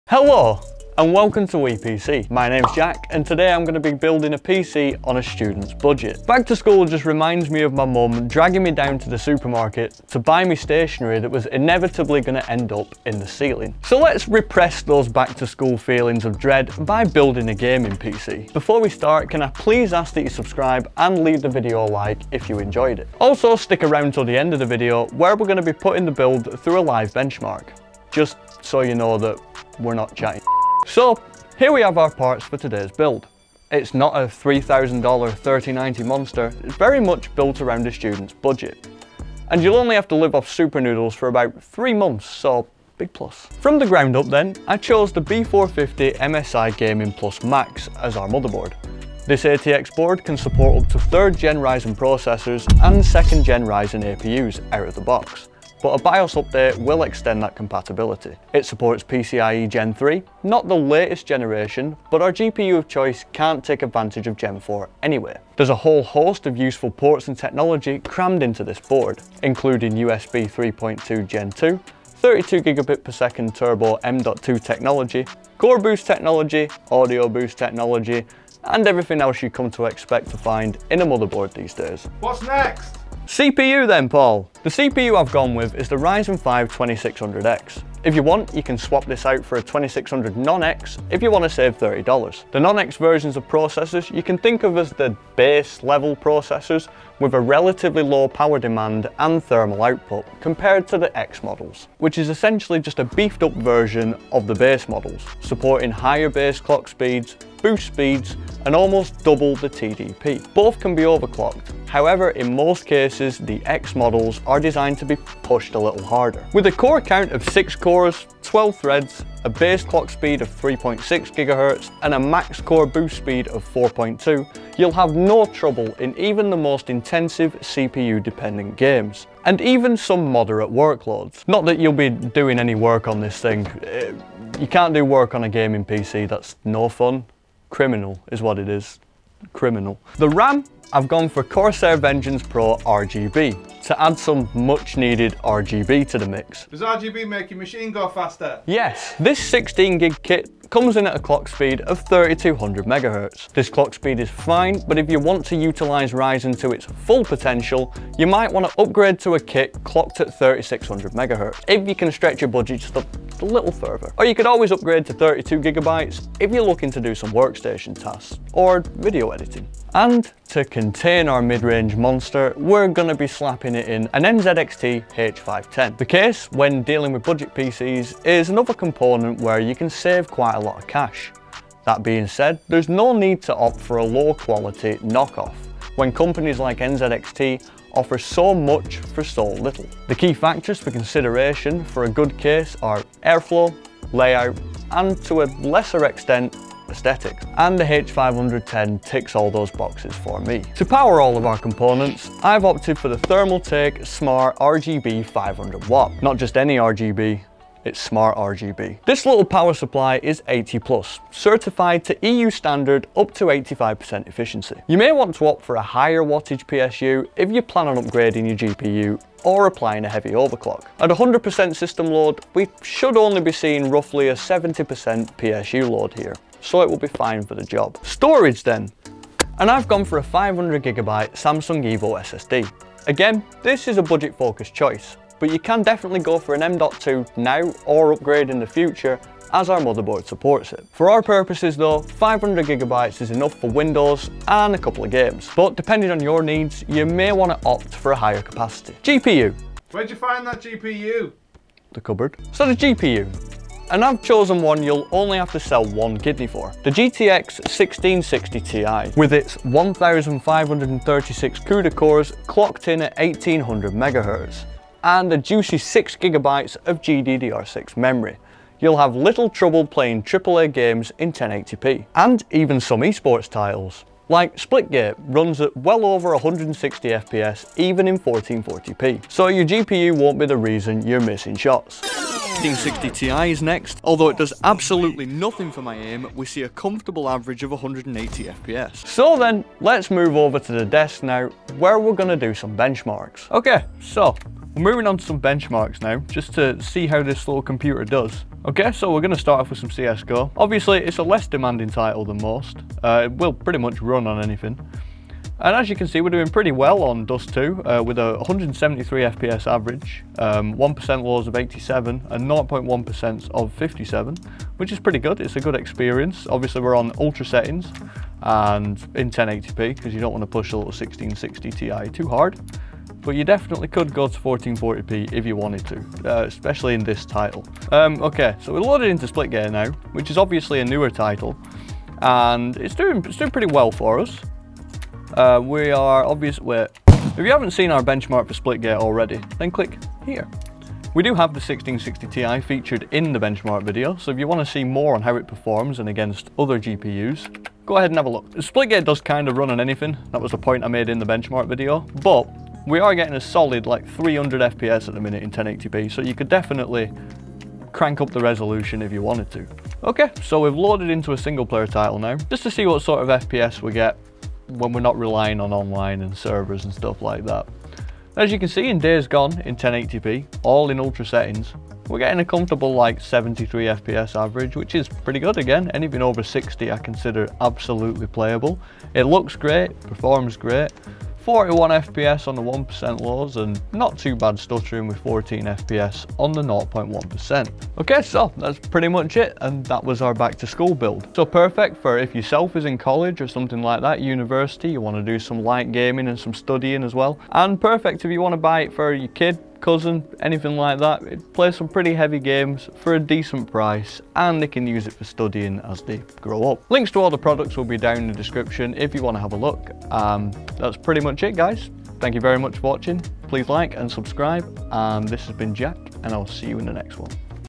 BEST $700 Back To School Gaming PC Build!
Today Jack brings you a back to school Gaming PC build! Perfect for studying as well as gaming! At the end of this PC build, Jack provides benchmarks to show you the incredible performance that this machine can offer!

🔍 Find all the parts used in this PC Build here:
➡ MOTHERBOARD // B450 gaming plus max
➡ CPU // Ryzen 5 2600x
➡ RAM // CORSAIR VENGEANCE 3200Mhz
➡ CASE // NZXT H510
➡ PSU // Thermaltake Smart RGB 500W
➡ SSD // Samsung 860 EVO 500GB
➡ GPU // GEFORCE GTX 1660ti